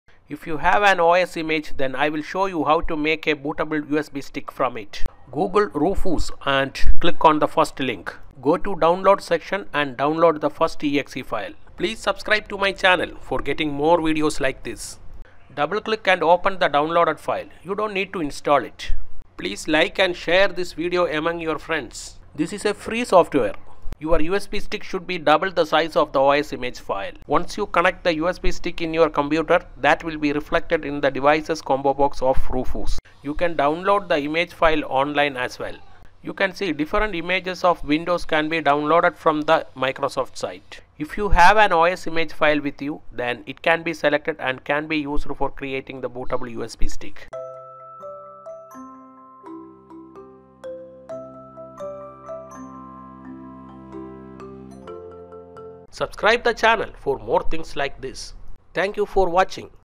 Create Bootable USB Stick
This video will tell you how to make bootable pendrive using free software